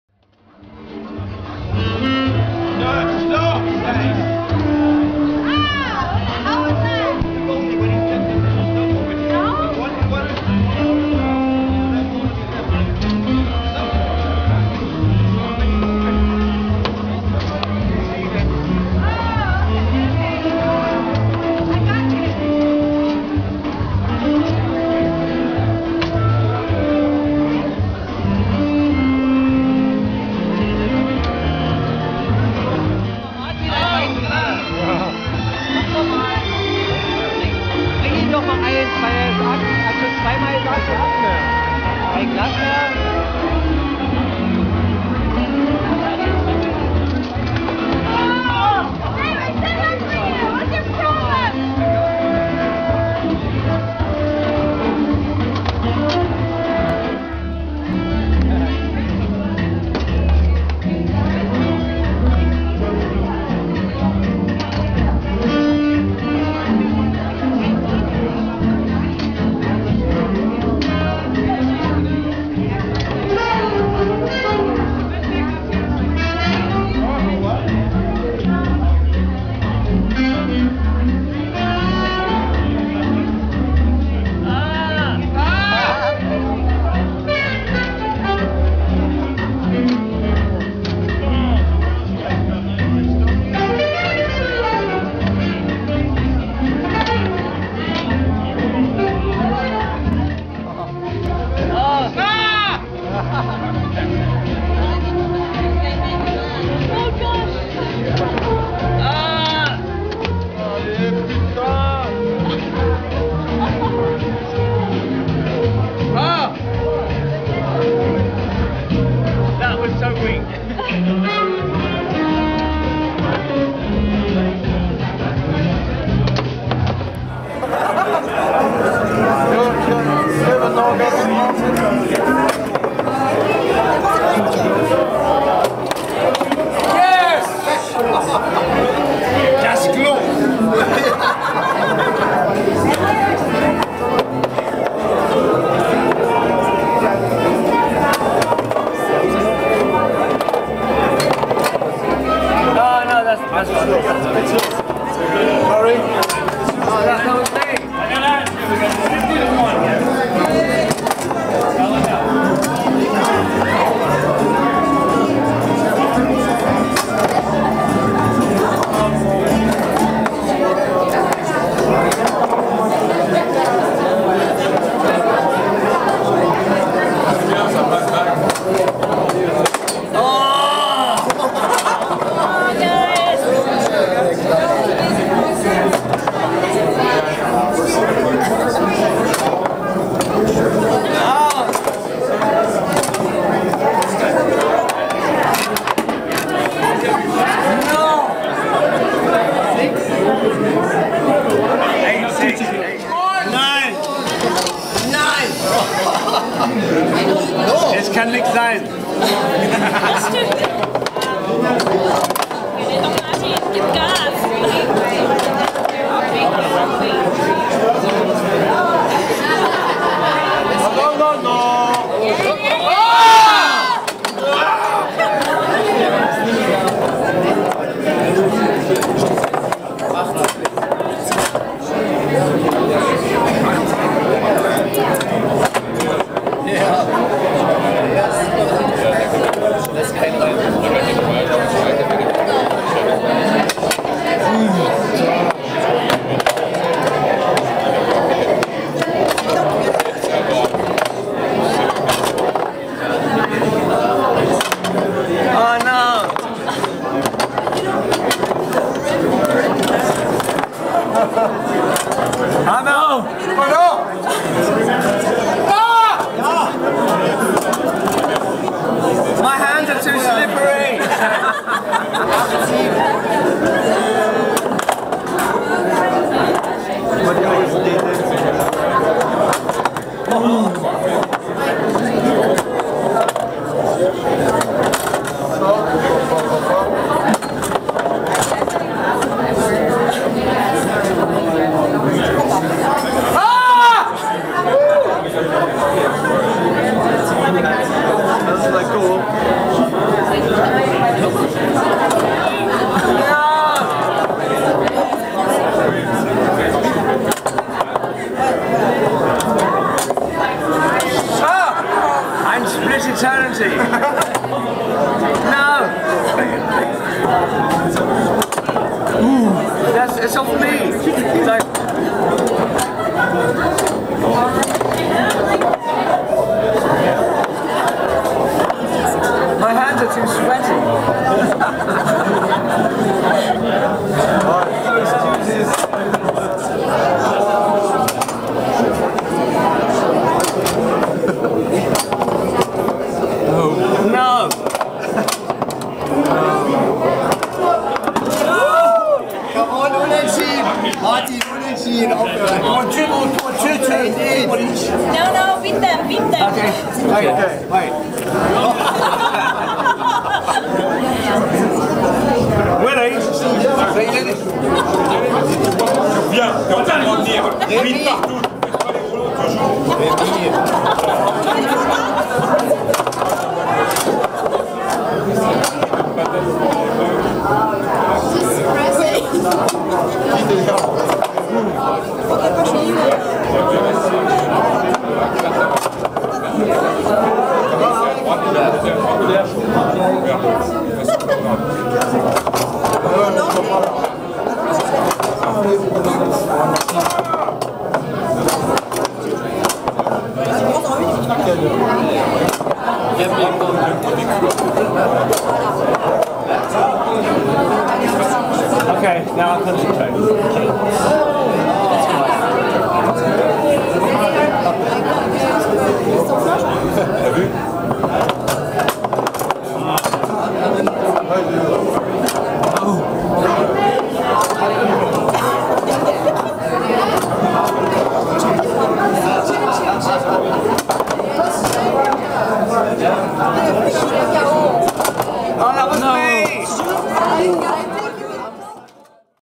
Martin Gore | BACKSTAGE in PARIS 22./23.02.2006
Martin Gore plays tablefootball with fans and peter gordeno "backstage" after second concert in paris 2006